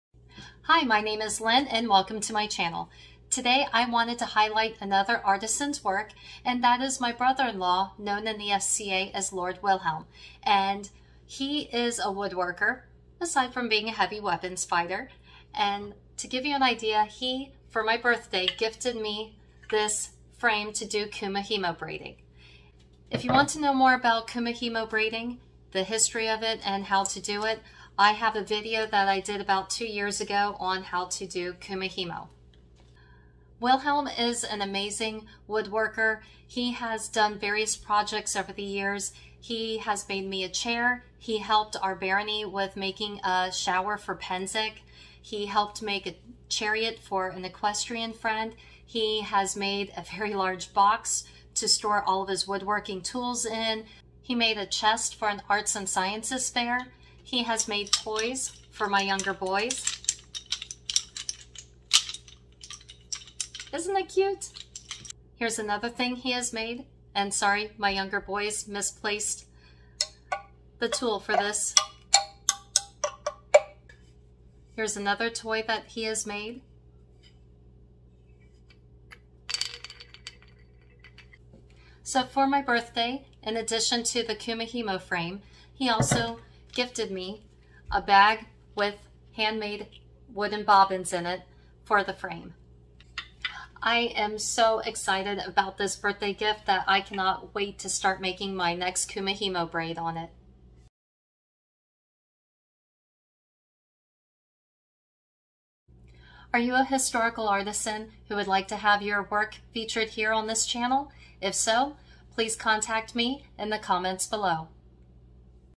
Featured SCA Artisan of the Week: Lord Wilhelm, a Carpenter
Carpenters, also known as woodworkers, have been around for centuries! For this week, I would like to feature (as he's known in the SCA) Lord Wilhelm's fantastic work.

Among his many woodworking projects, Lord Wilhelm has made a foldable chair, a chest, a decorative box, a showerhouse for Pennsic, and so much more!

Are you a historical artisan who would like your work featured on this channel? Please let me know!

For anyone curious about what exactly the 'SCA' is -- The Society for Creative Anachronism is an international, non-profit educational organization that studies and recreates the medieval and Renaissance years of 600-1600AD.

For more information and to locate your local branch, please check out